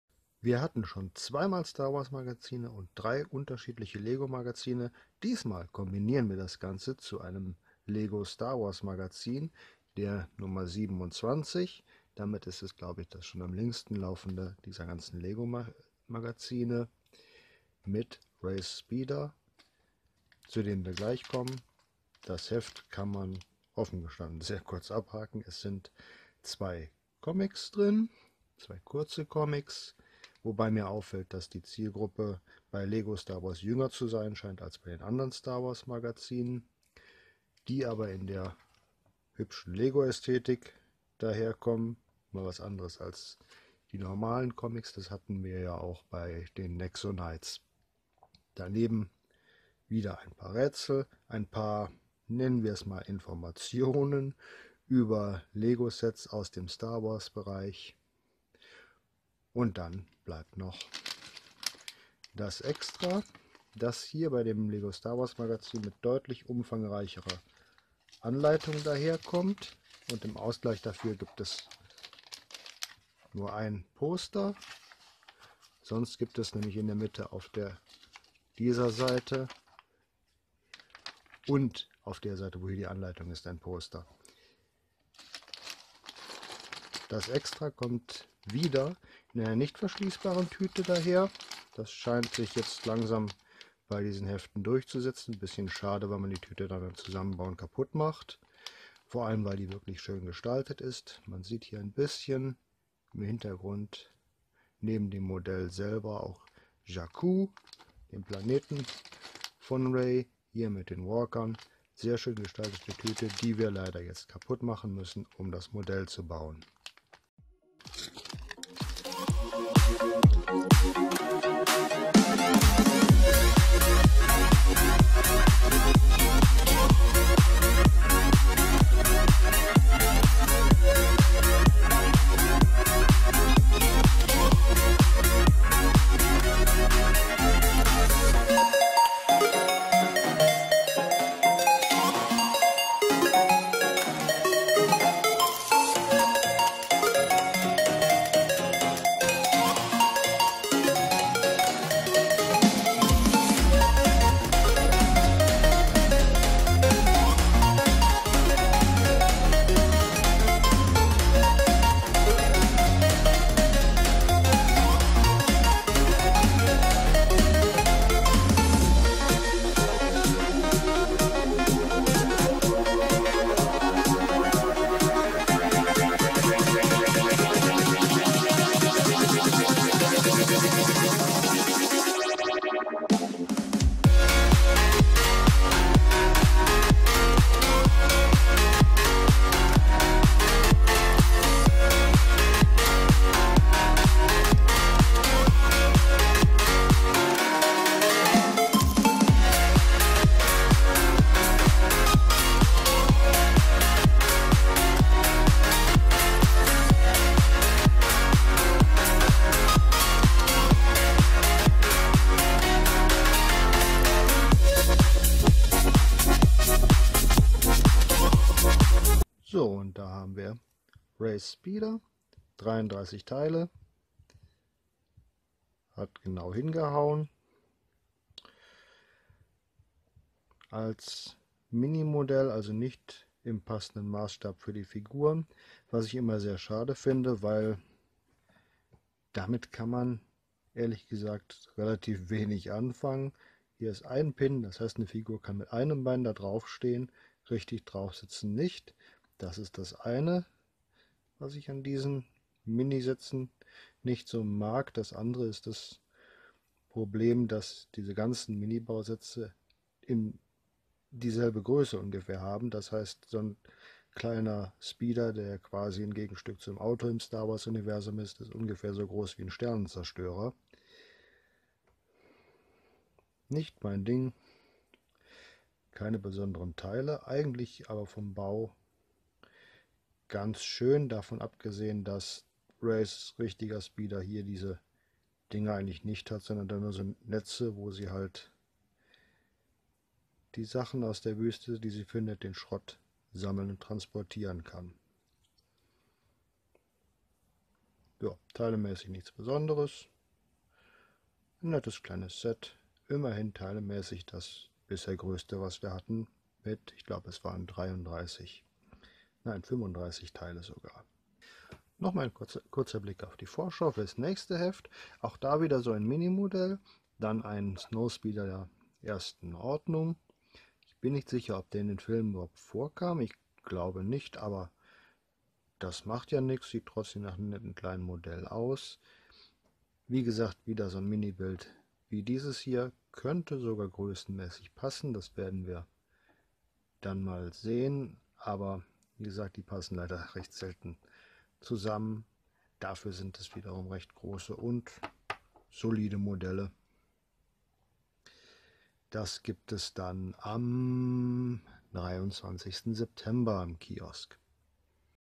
Lego Star Wars Magazin 27 mit Reys Speeder - Extra Extra Test
Erstmals ein Video im künftigen Format: Kurze Videos für ein Heft statt lange Sammlungen. Und es beginnt mit dem heute erschienenen Lego Star Wars 27 plus einem Minimodell von Reys Speeder-Bike aus Das Erwachen der Macht.